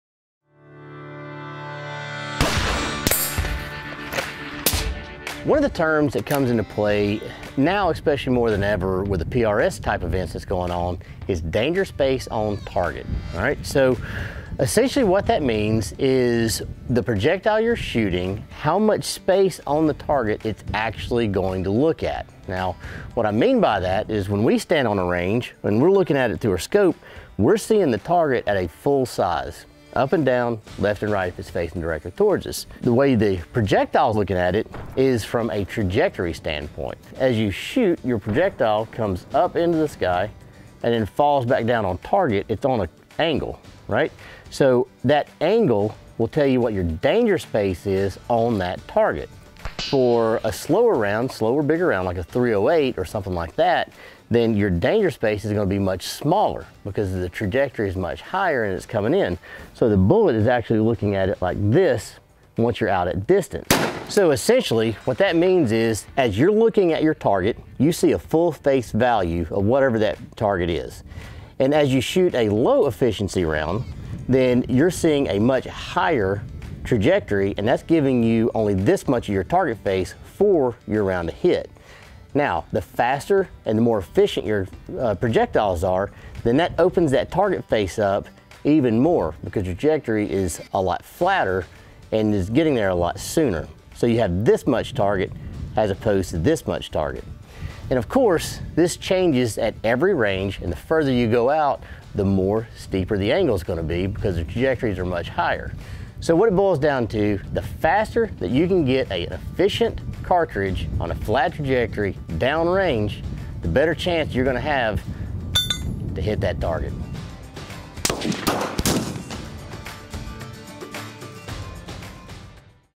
Danger Space Defined
When you push range to extremes, your target is not the size it appears to be. Federal ambassador Jim Gilliland explains the concept of danger space and reveals how cartridge choice plays a role.